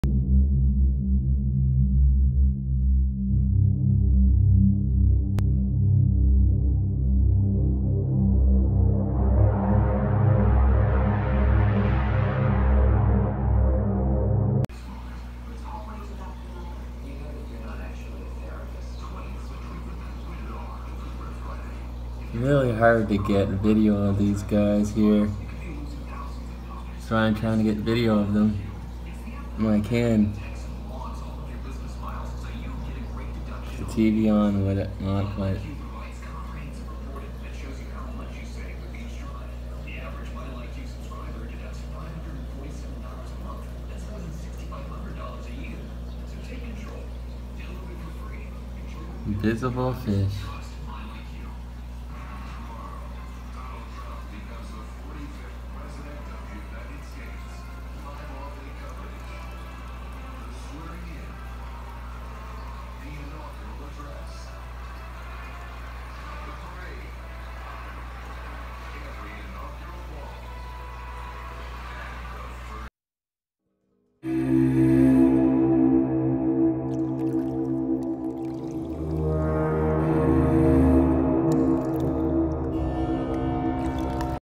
Invisible Fish
This is a look at my glass fish  !!